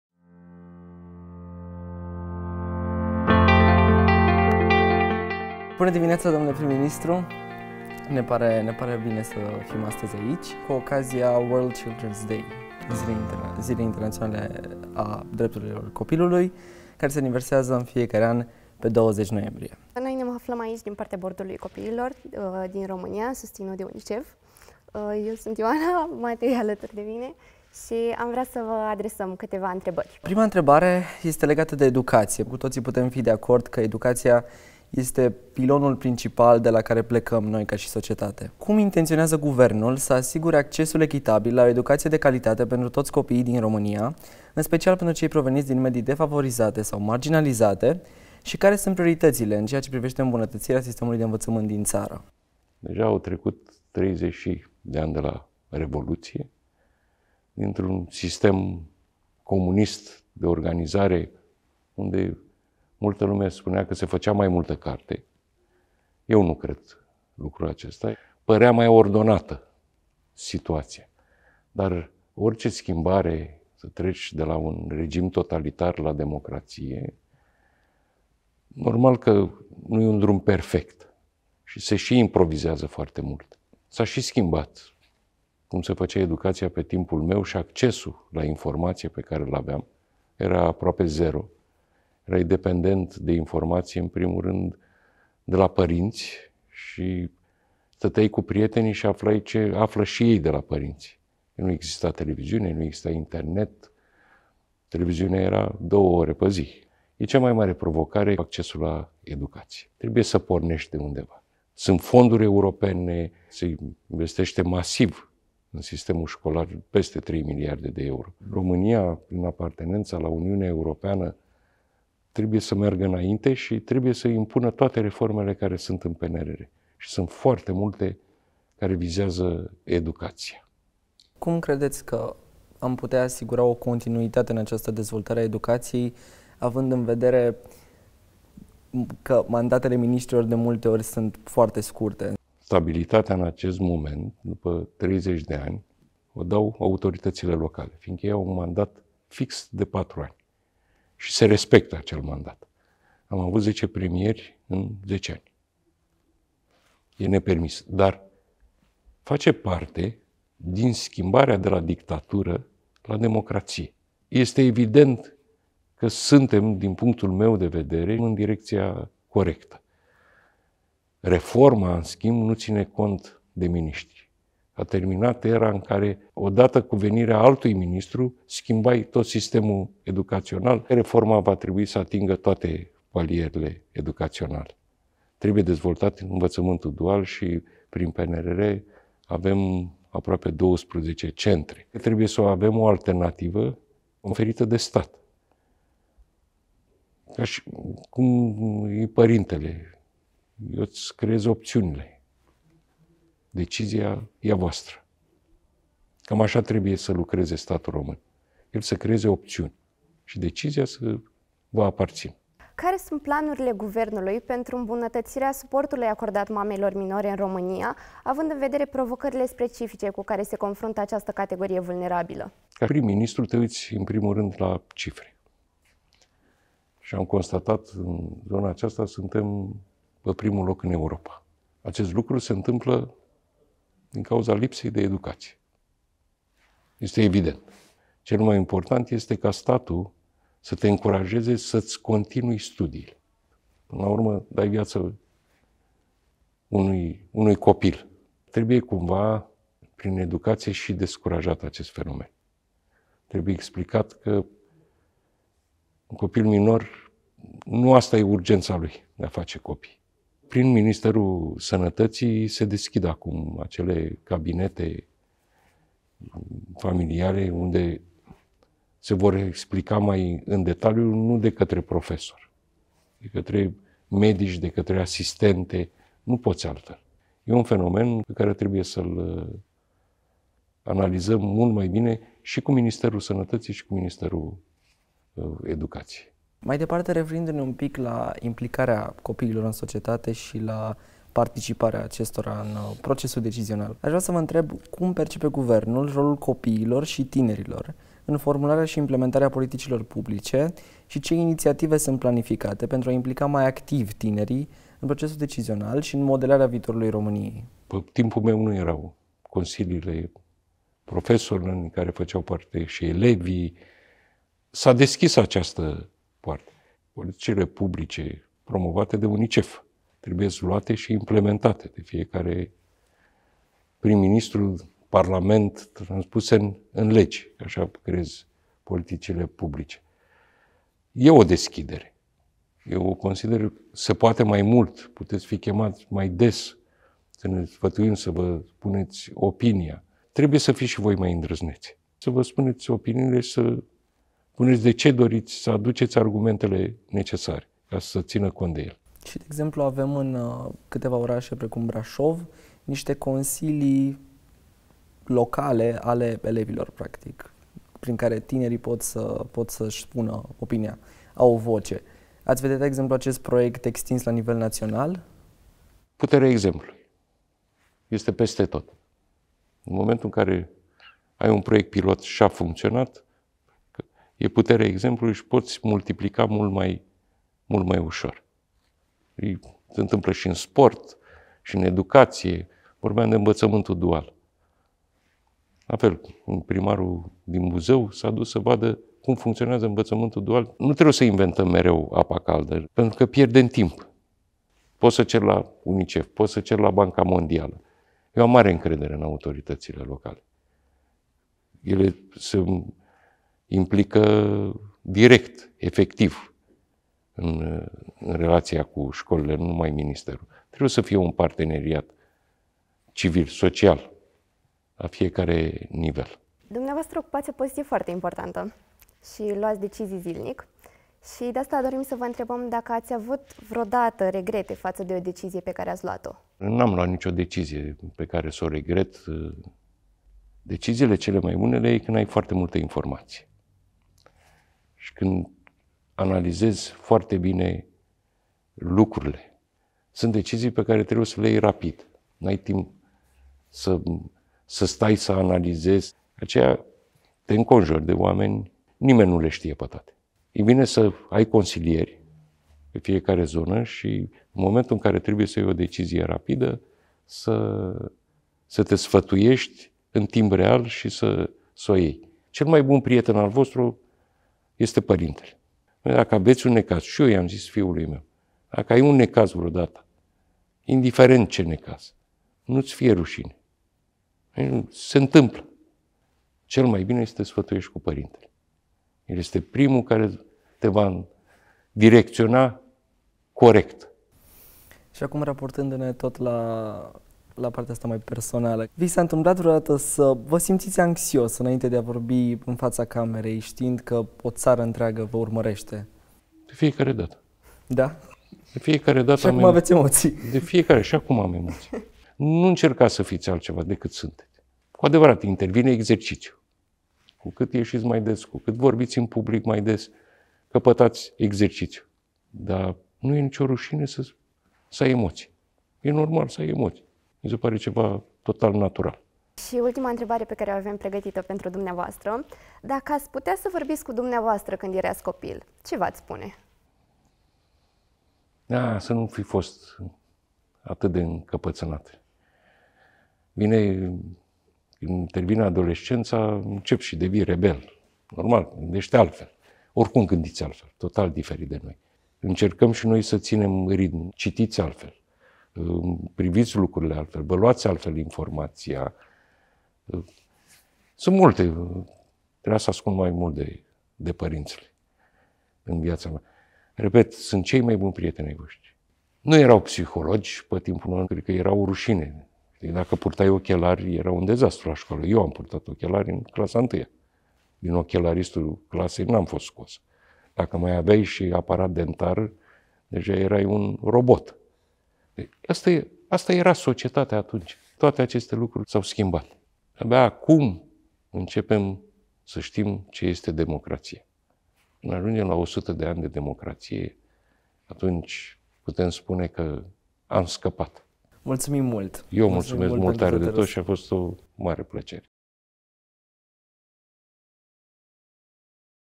Children take the lead in interviewing Romanian Prime Minister on WorldChildrensDay 2023
On WorldChildrensDay, Matei and Ioana from the Children's Board engaged in an exclusive interview with Romanian Prime Minister Marcel Ciolacu, discussing the future of education, support for adolescent mothers, and the active participation of children in decision-making.